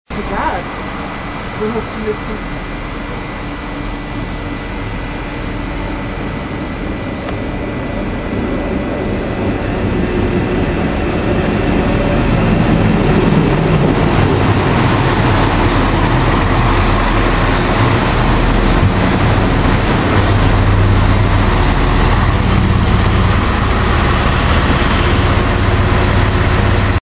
BR 642 im Bahnhof Dessau Hbf
642 XXX kommt als RB50 aus Richtung Güsten in den Bahnhof Dessau Hbf eingefahren